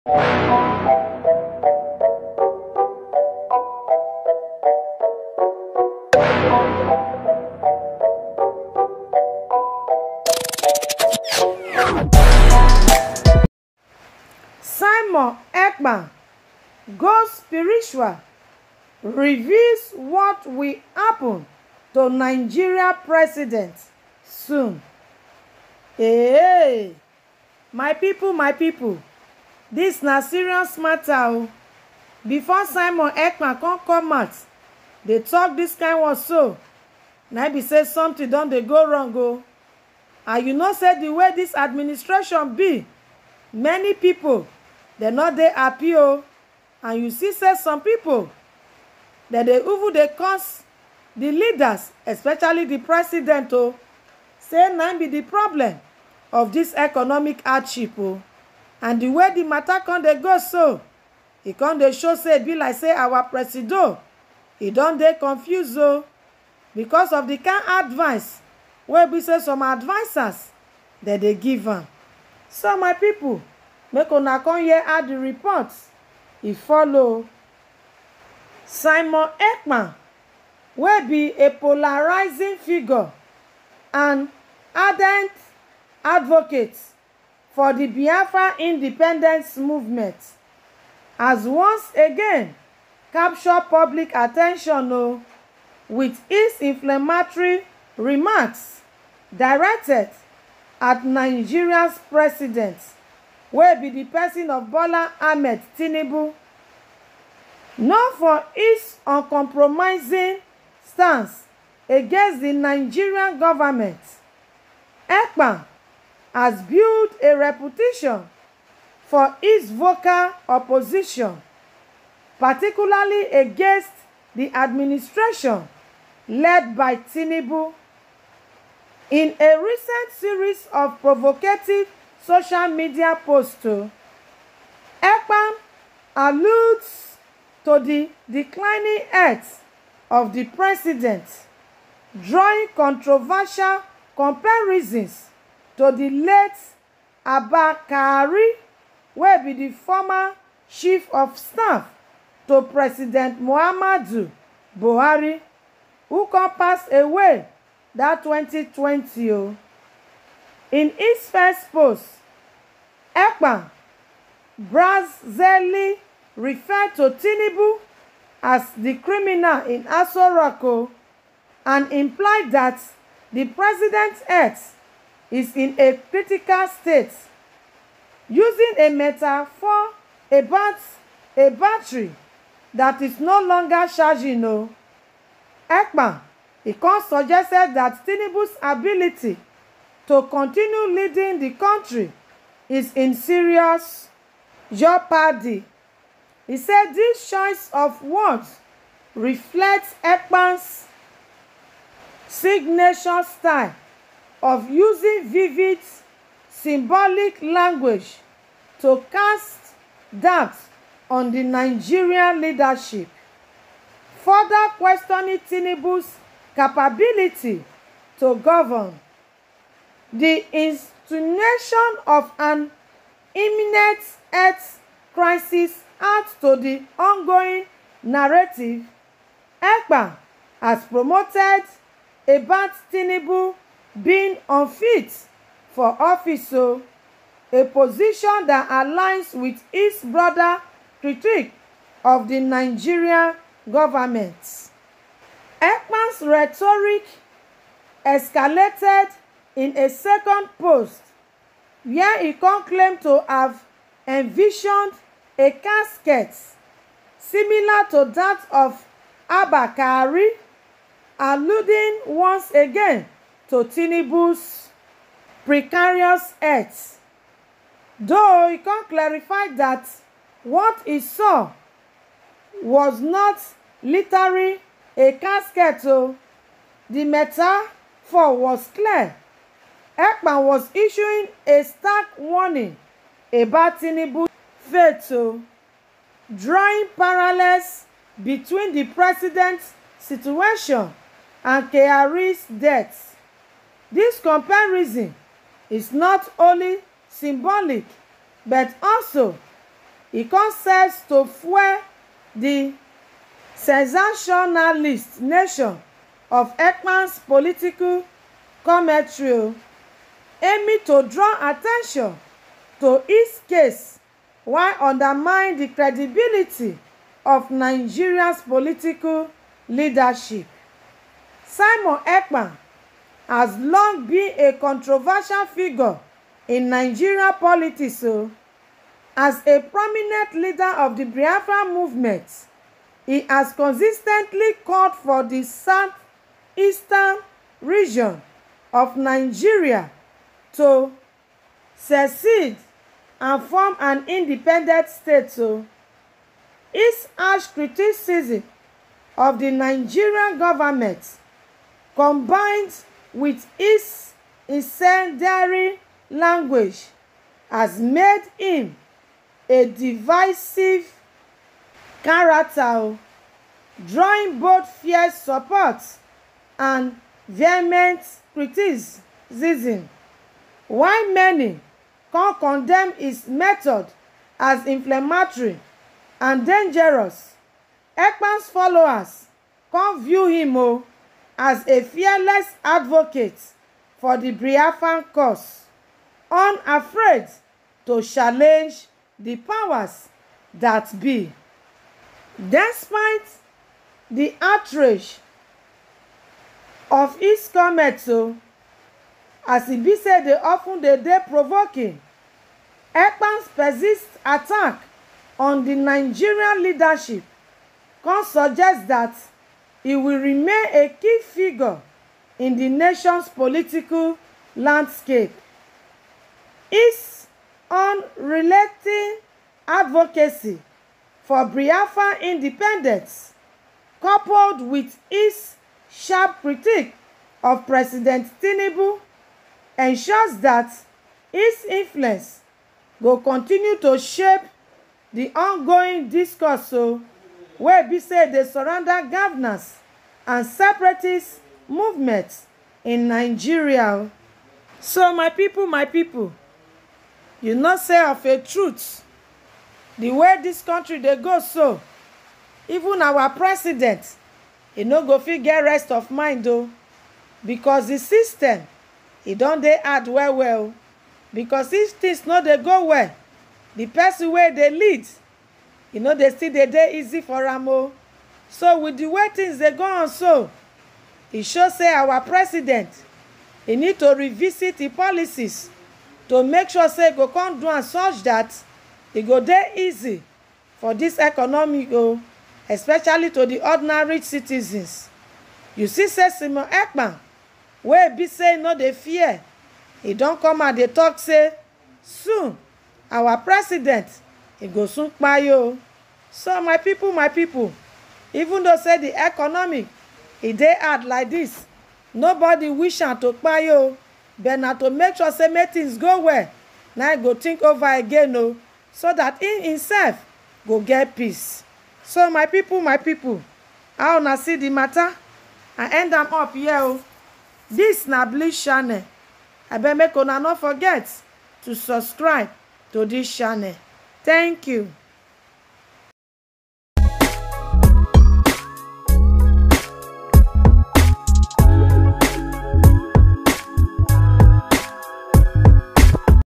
Simon Ekpa Makes Shocking Statement, Reveals What Will Happen To Nigeria President Very soon
Simon Ekpa#Tinubu